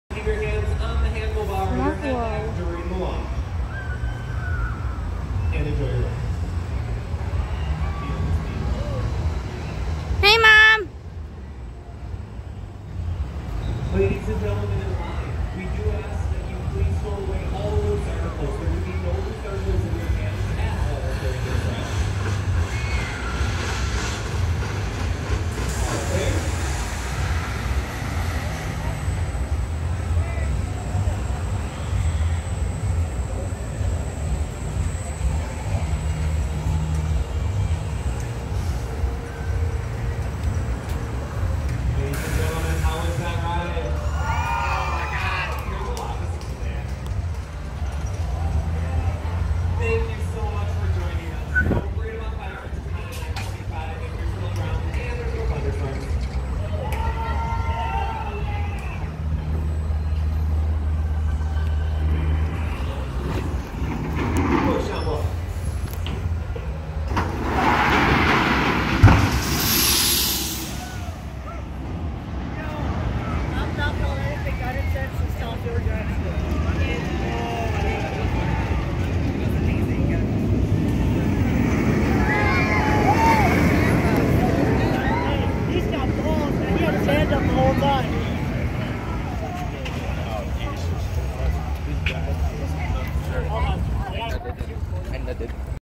MaxxForce ....7/5/19 ...LOVED IT🥰
opening weekend of Triple record breaking Launch coaster at Six Flags Great America